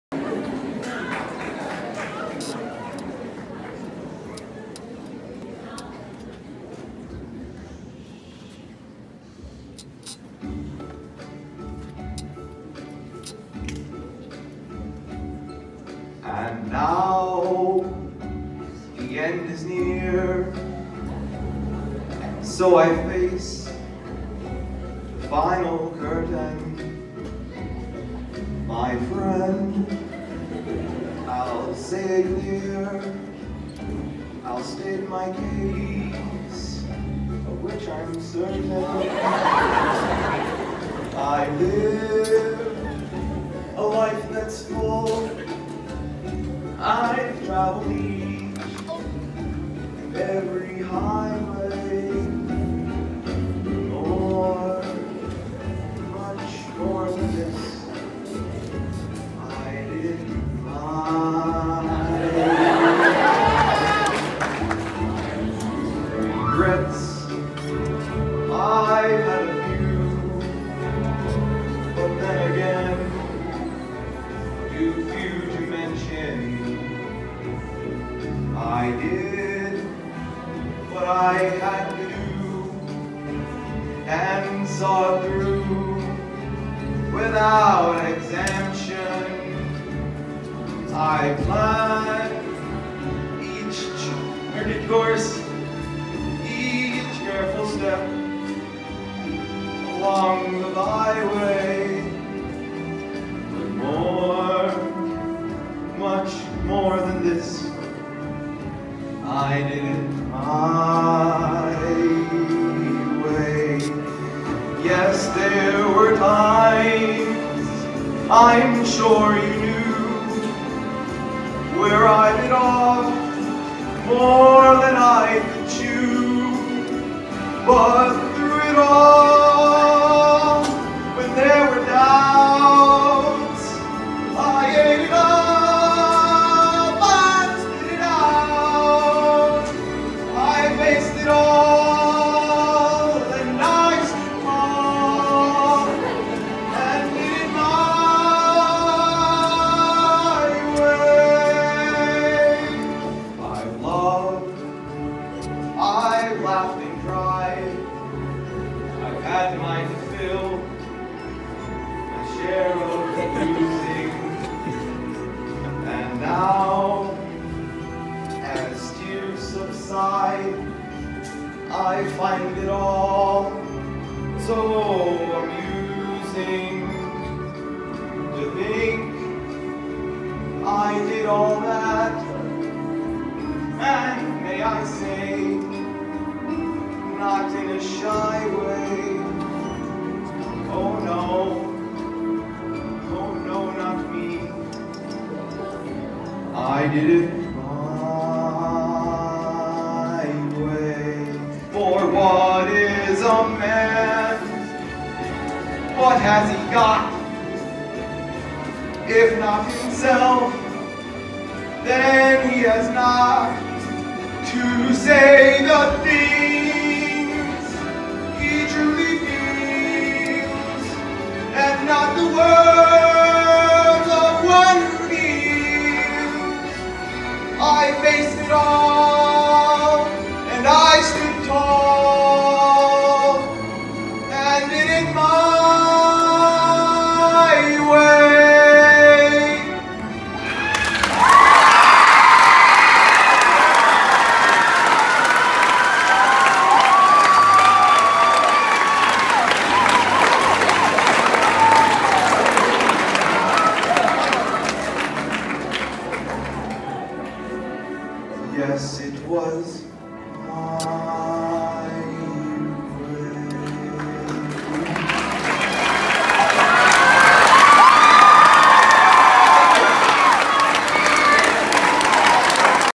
Tuck McKean singing Frank
2018 Wauseon Middle School talent show